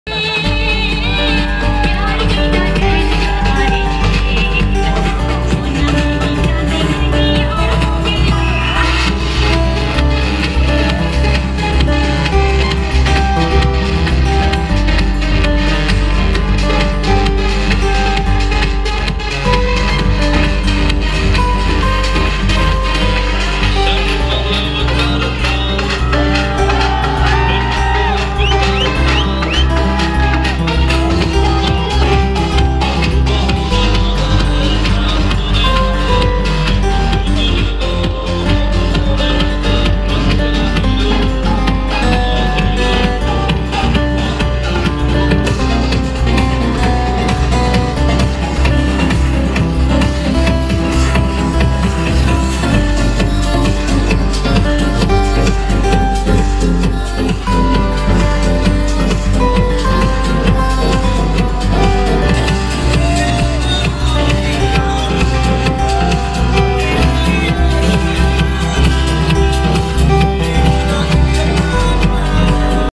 Aiman Khan and Muneeb butt Dancing Like Crazy at their Mehndi Event
Watch Exclusive Video of Aiman Khan and Muneeb butt Dance Like Crazy at their Mehndi Event